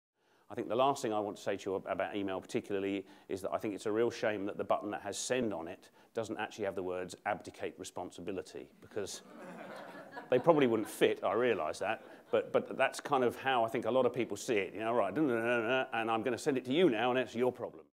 Abdicate Responsibility
Doug Shaw talking about making work better at Louisiana SHRM 2013